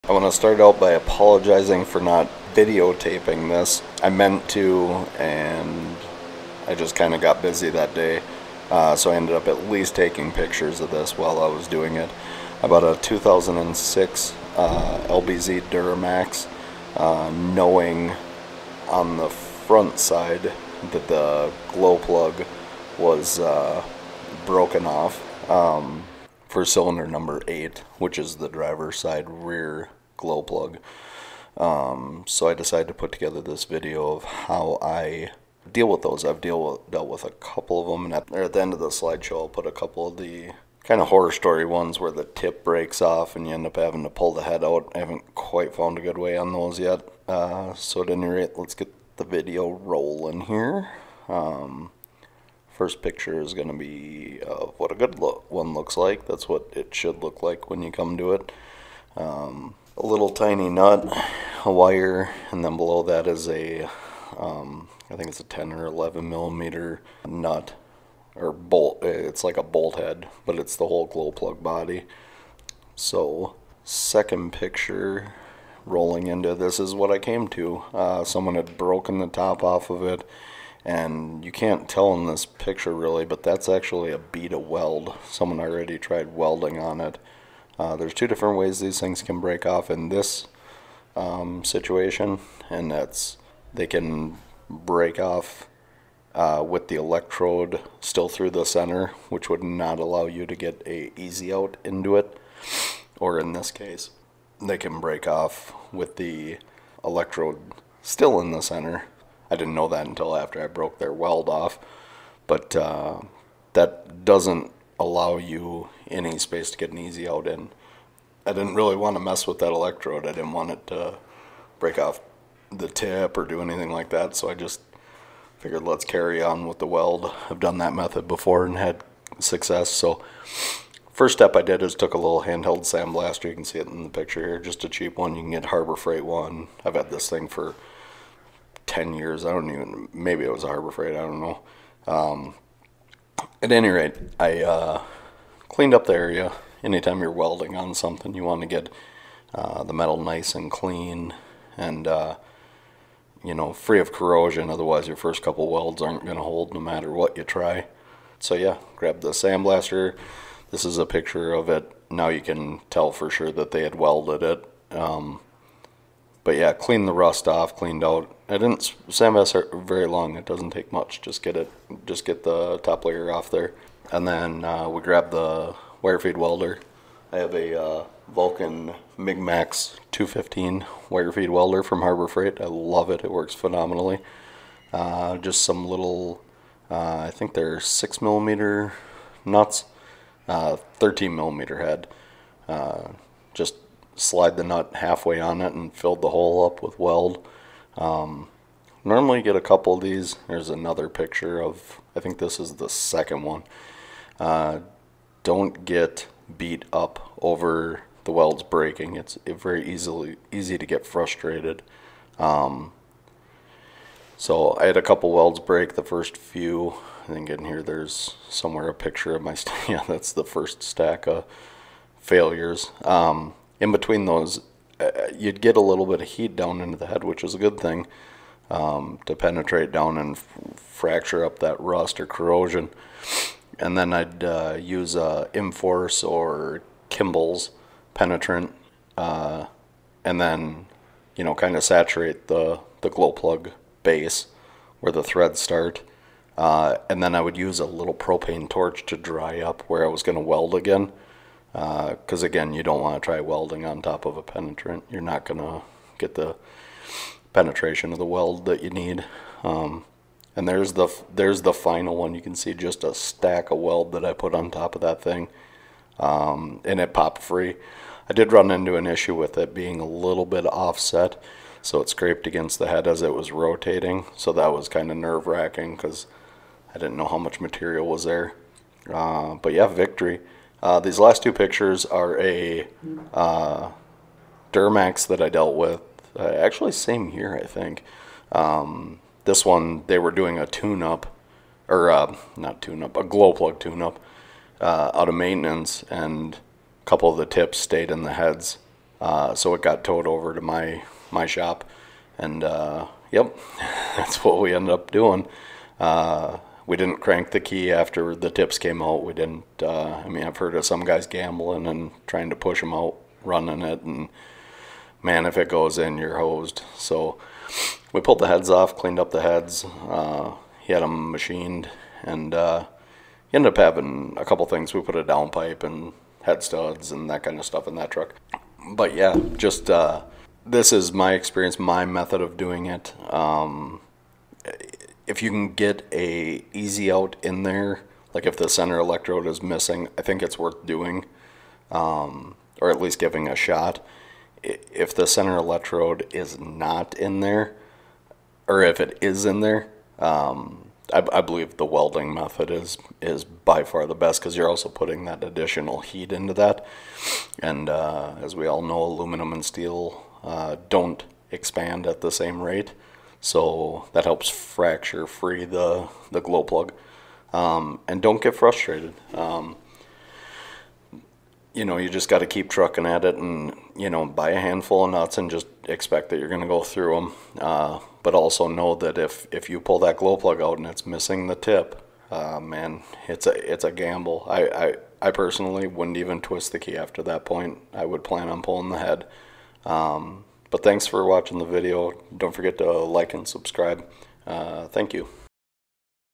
LBZ Duramax Broken Glow Plug Removal
just my method of extracting broken glow plugs on the Duramax 6.6L. LB7,LLY,LMM,LML should all be very similar to this.